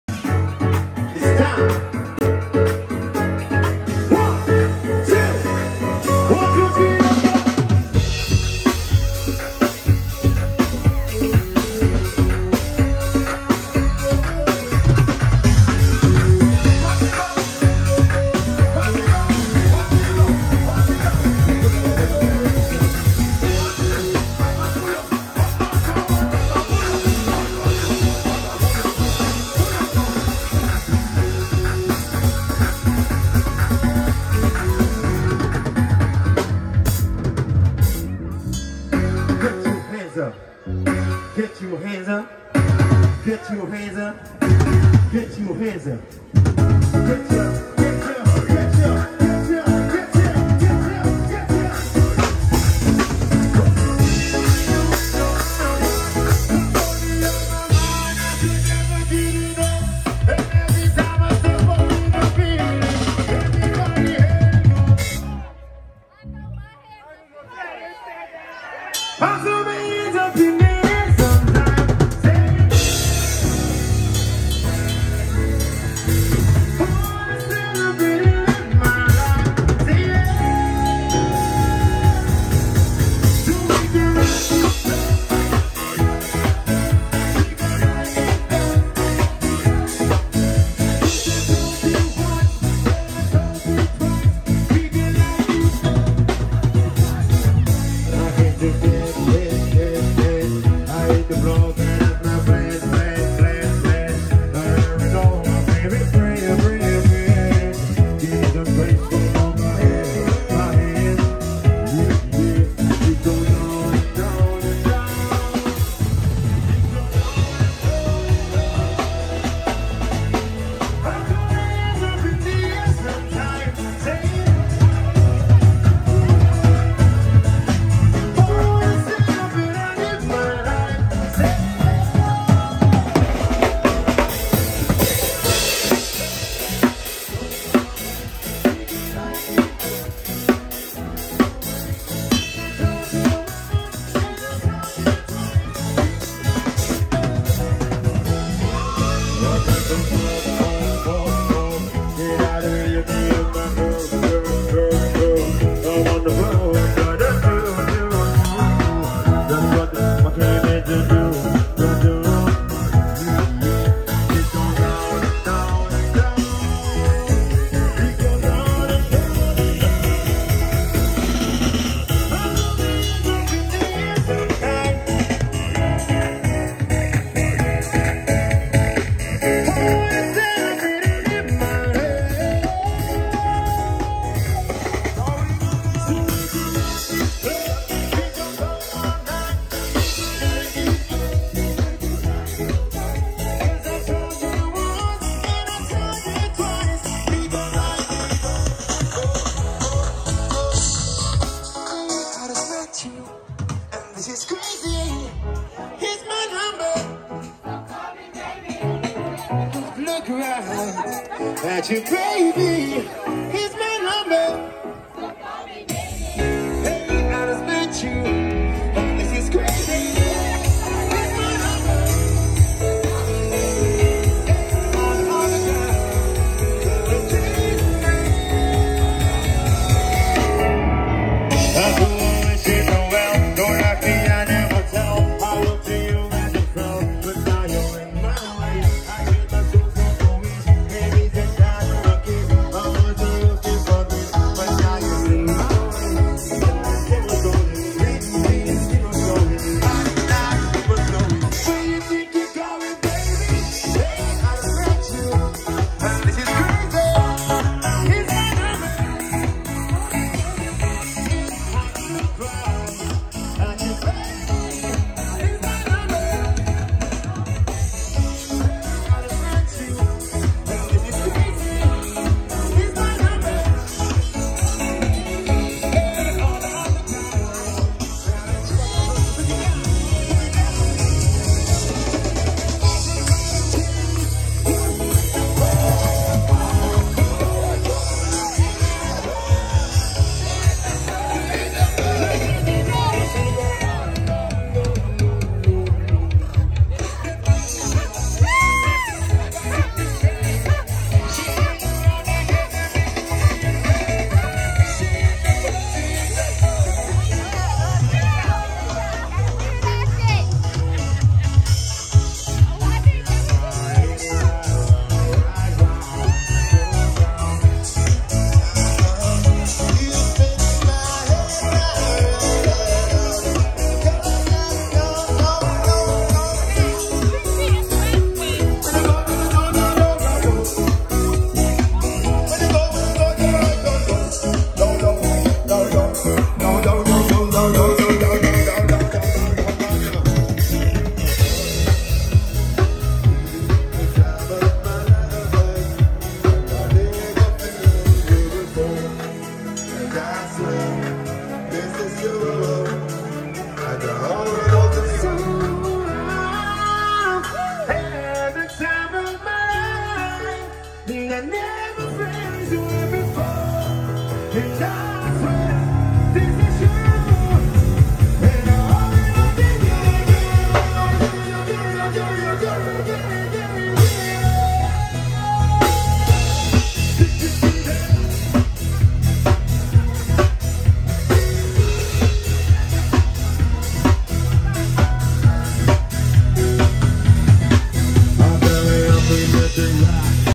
greenhouse band - New Year Soiree
greenhouse band greasing up the dance floor while ringing in 2022 at the Four Seasons Resort Nevis.